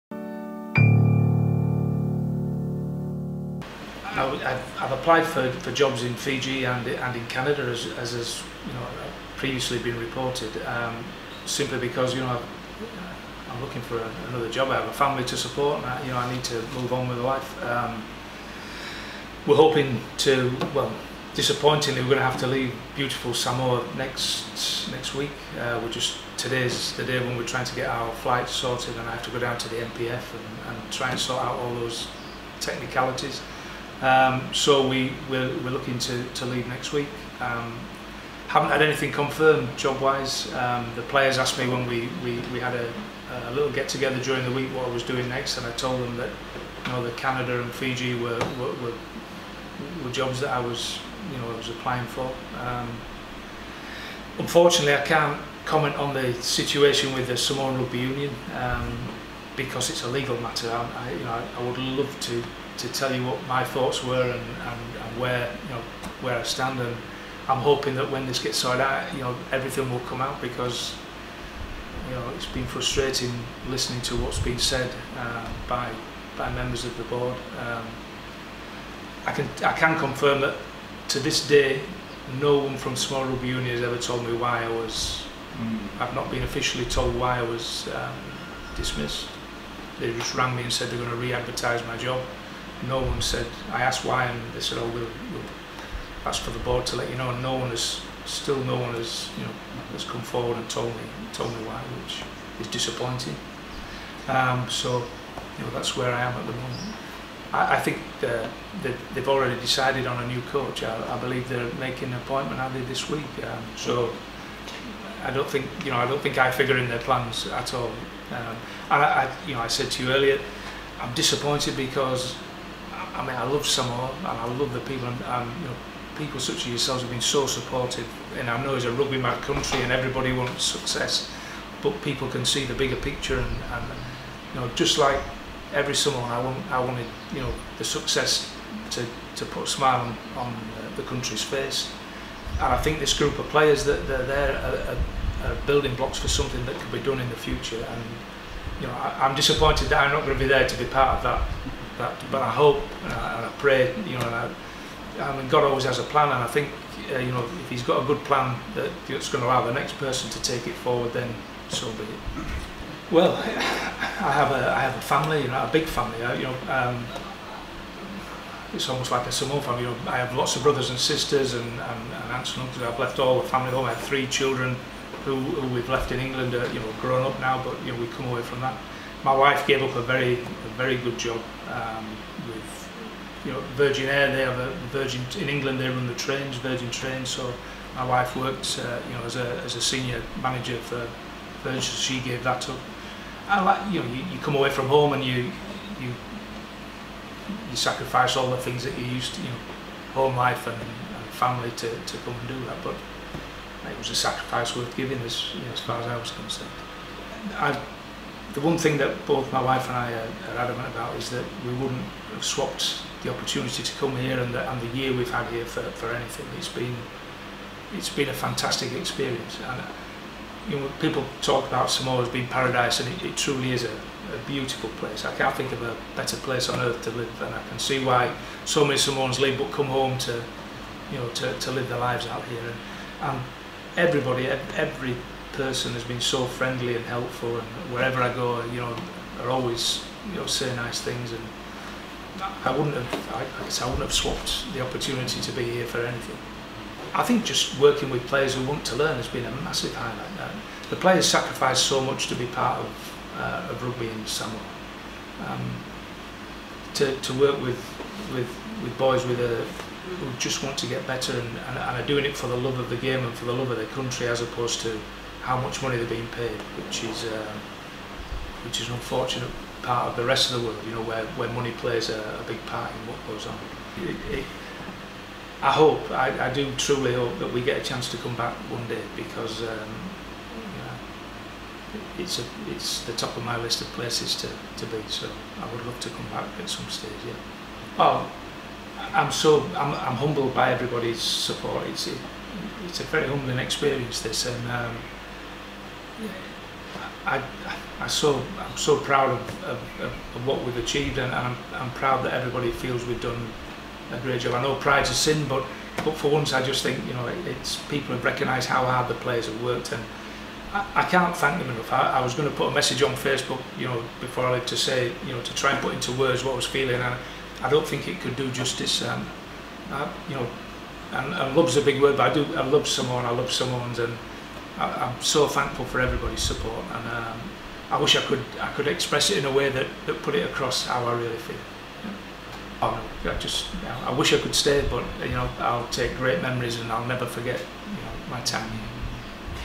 Coach breaks silence
The Samoa Rugby Union (S.R.U) has not had the courtesy to explain to Damian McGrath why he has been sacked.